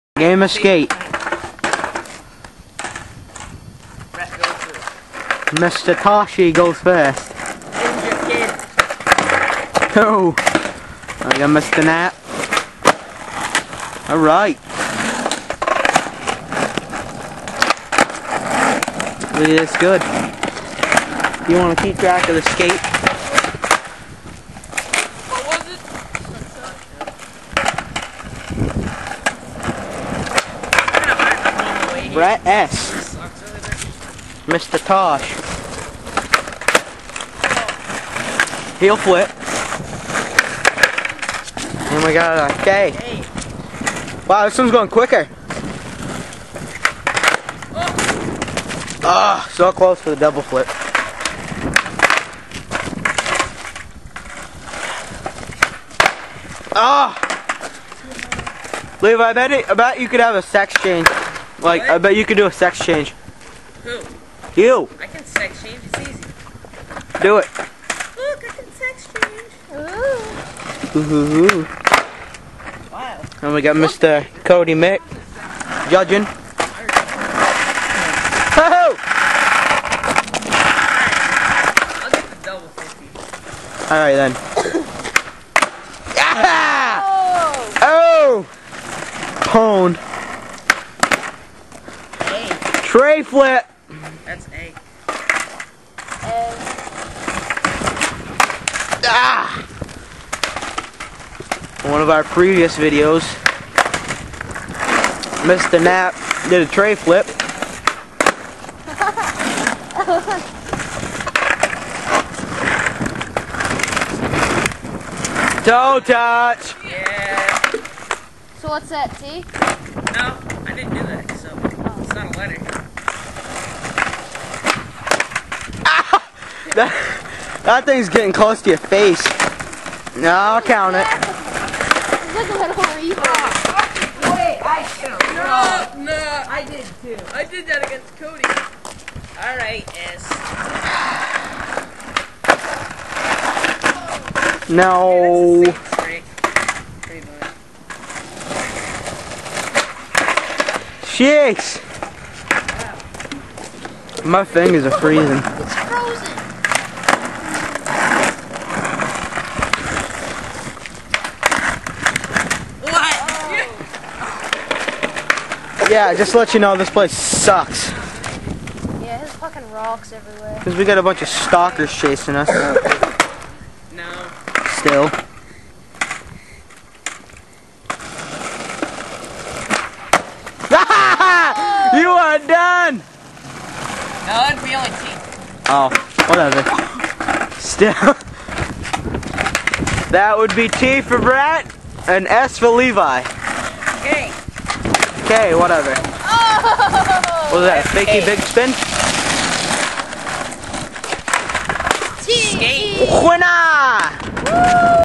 Me Vs Brett Game Of Skate (Not Part of the Tri Town Battle) READ DESCRIPTION
there is a fat girl and a gay kid who keep following me and my friends. they have been following us for the last couple of days when we go downtown to skate. so we noticed them across the street when we went to Cumby's
to get a drink. so we hid on the side of cumby's and waited til they went by. so we skated to the water district and
they were across the street from there and they noticed us. so they came over and we ran from them up to union street and skated there. and they must have thought we went to the skate park so we were safe from them
for the rest of the day. haha. and that is why it was filmed at this
spot and not the skate park.